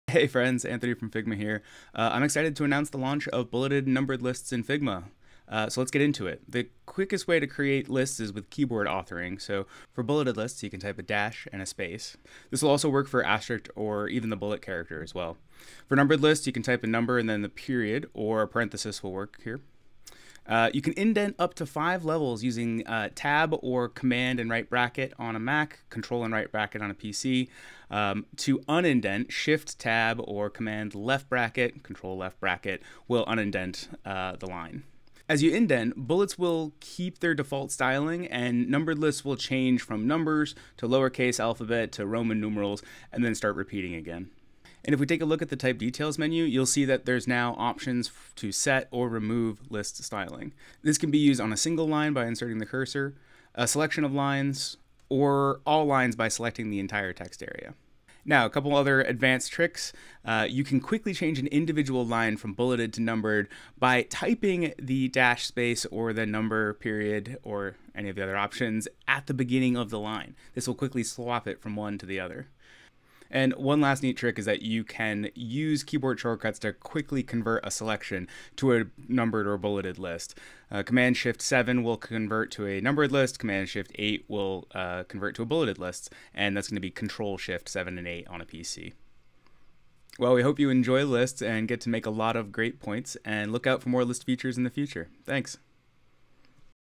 Bulleted and numbered lists in Figma
Lists are now supported in Figma! This video will cover the basics and more advanced tips to get the most out of bulleted and numbered lists.